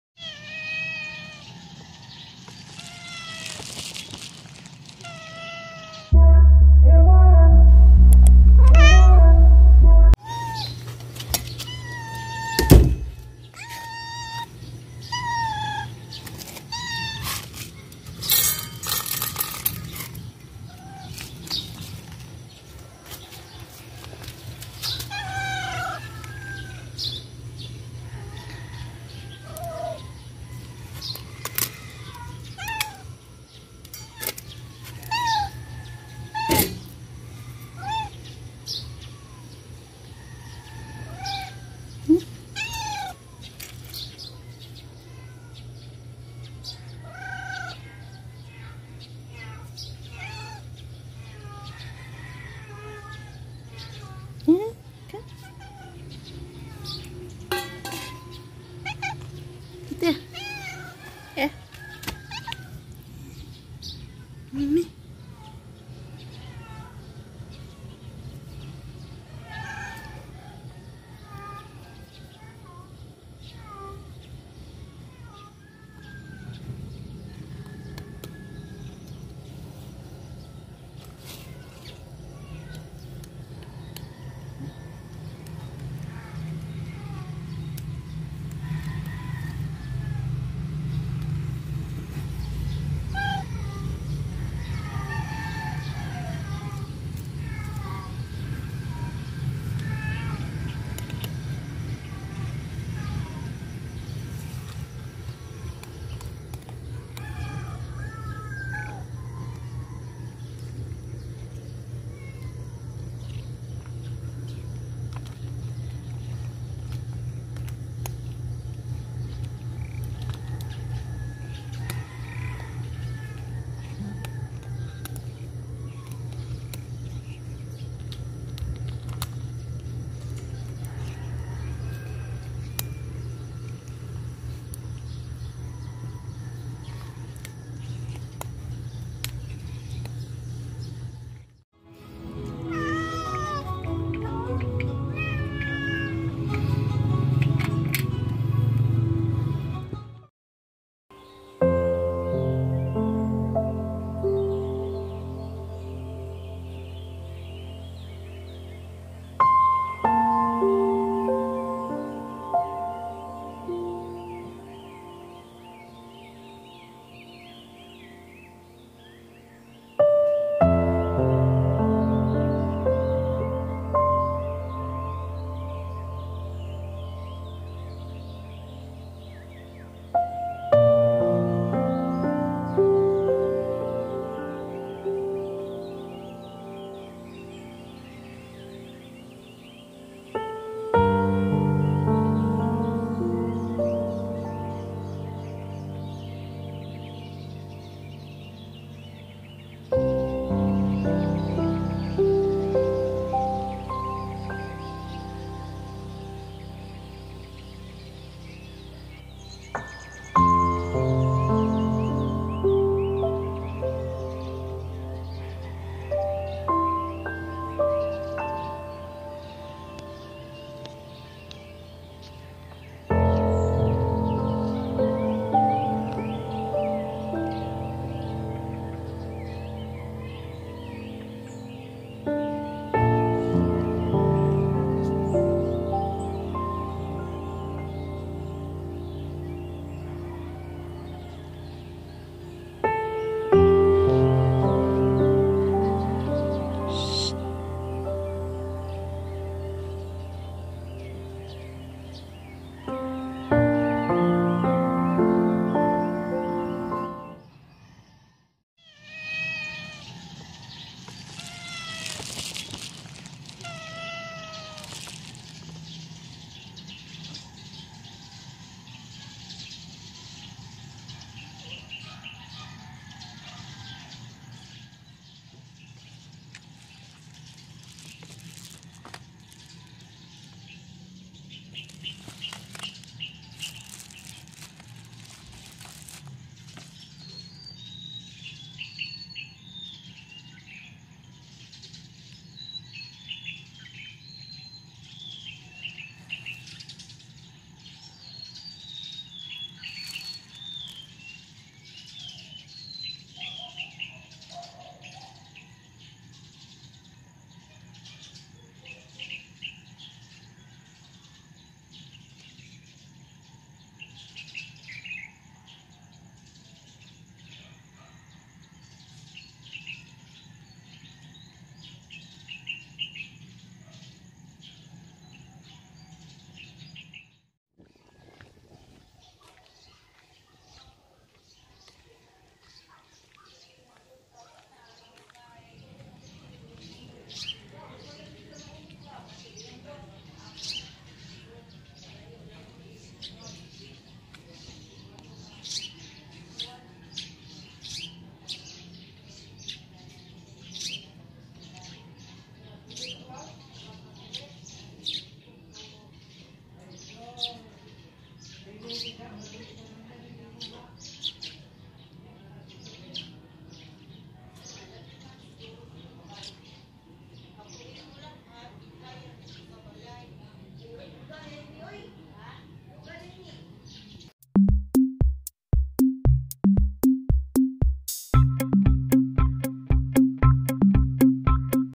Feeding beautiful stray cats and I found one of my rescue kitten's father.
There's another new Maui outside in our back door. She is pregnant too omg! But we cannot bring her in.
So many are asking about the cats that we have fed before from our previous house.
They want to build something there and all the cats are gone. I walked around to find the cats but i couldn't find them. In the past almost everyday i went there to find the cats but i failed...One day i found out the cats that we fed before, they went back to their previous hooman who feeds them. I am very happy to know that they went back and someone's feeding them.Do you all know that everytime i went there i cried a lot when i came back home..The reason why i don't like to upload some of their videos because it makes me sad...I need to move on because i have 9 cats to take care everyday now.We don't have any animals society or animals shelter association here in our place.It's very sad...Some people love them but some hate them. Unfortunately we can't help all cats, but we will do our best.

Anyway i found looks like Leo our friend 'The Leo's day off 'channel He is a male cat and loves to roam around near to their house.In our place animals can roam around but just have to be careful not to be hit by a car.

feedingstraycat pregnantcat hungrycats catslifeph# adorablecatsvideos#catvideos2020

Video Editing : Kinemaster
Camera Filming:Samsung Note 10
Thumbnail Design: PicArt